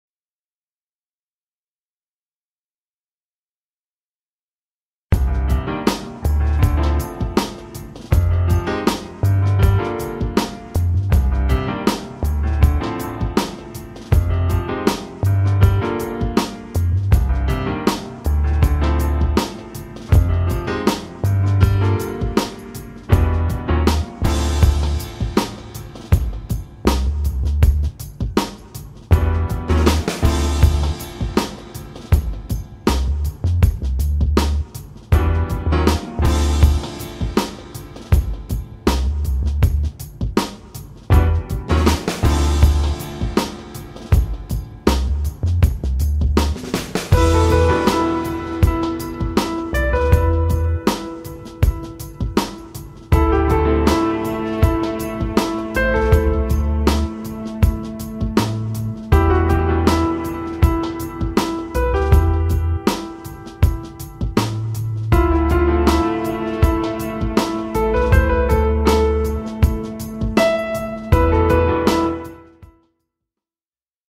Renault clio occasion visible à Albi présentée par Automobiles albigeoises
Cette Renault clio occasion disponible à Albi dans le 81 est mise en vente par Automobiles albigeoises - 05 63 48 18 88 - tous les détails sont